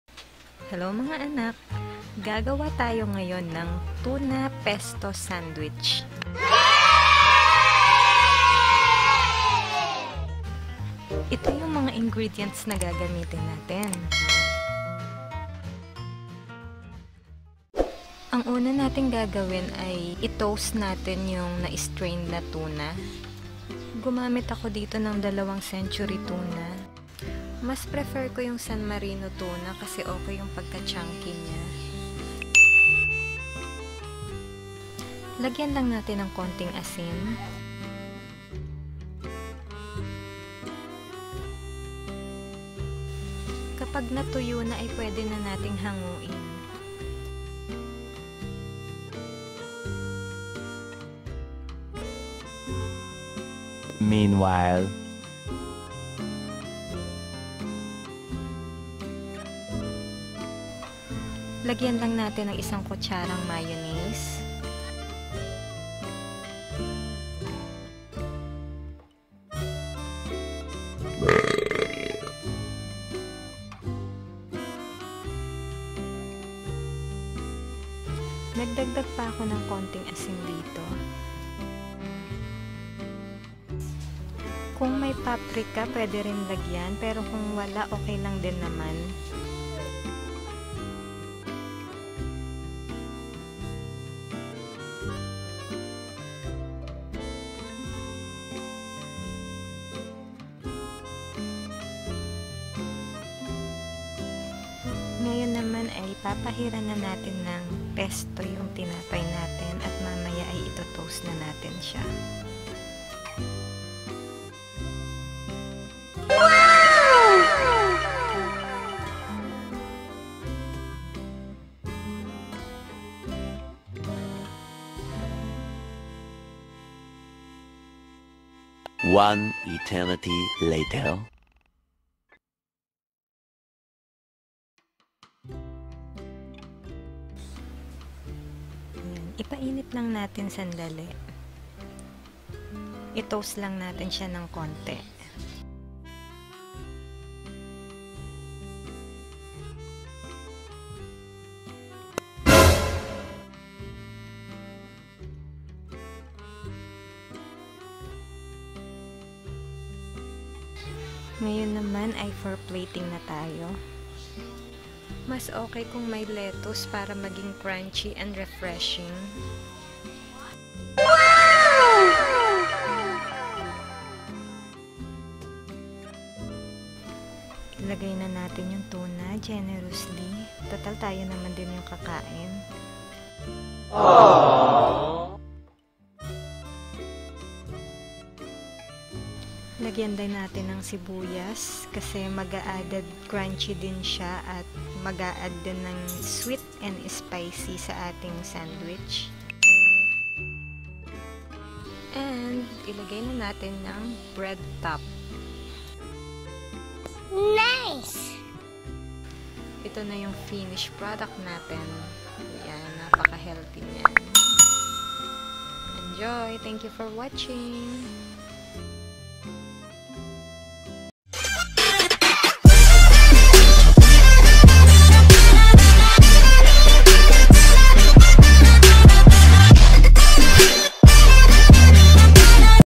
TUNA PESTO SANDWICH
Made Tuna Pesto Sandwich for my family.
If you are looking for a healthy snack, you can try this. :)